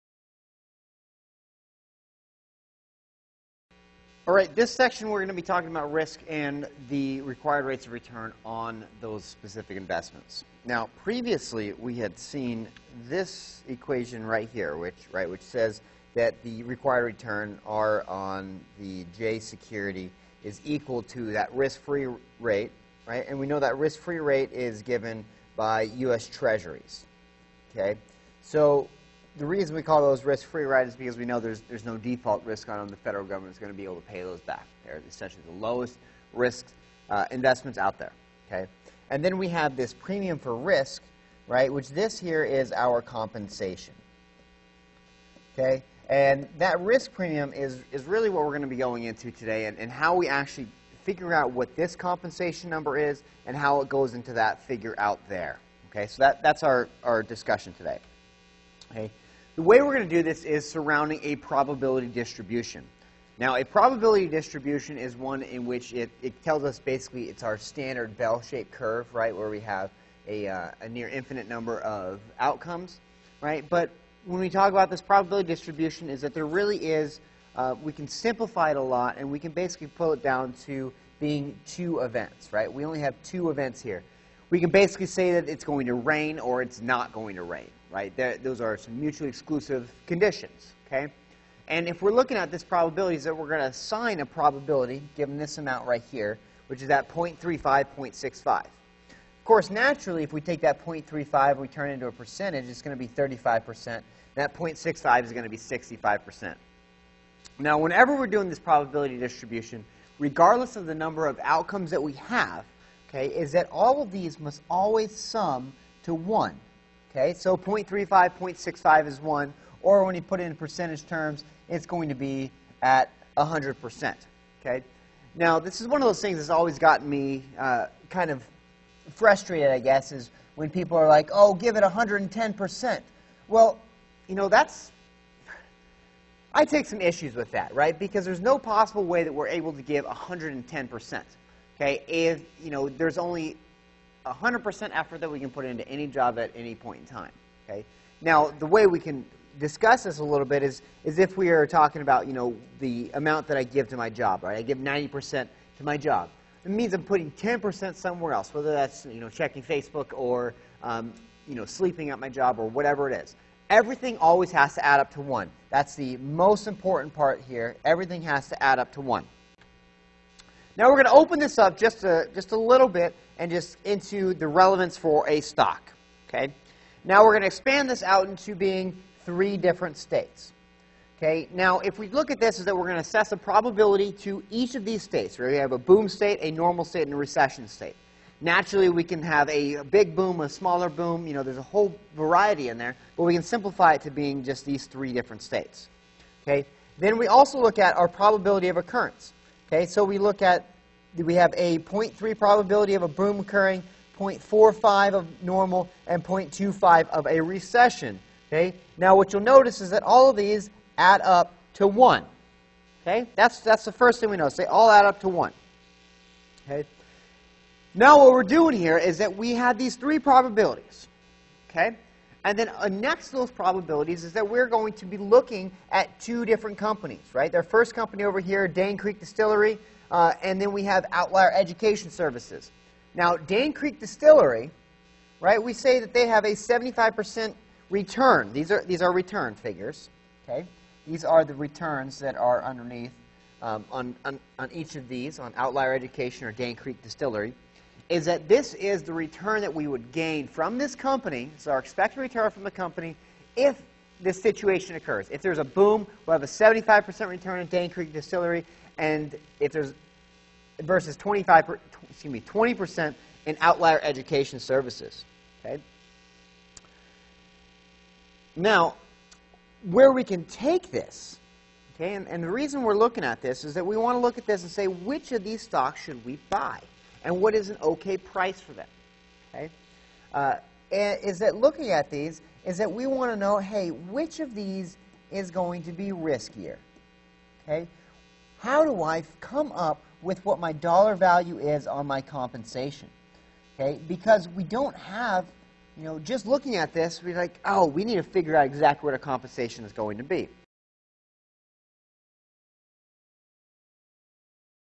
Risk Intro: Probability Distribution
Introduction to the section on risk and required returns.  Discusses what a probability distribution is and why its important

Video by Chase DeHan, Assistant Professor of Finance at the University of South Carolina Upstate.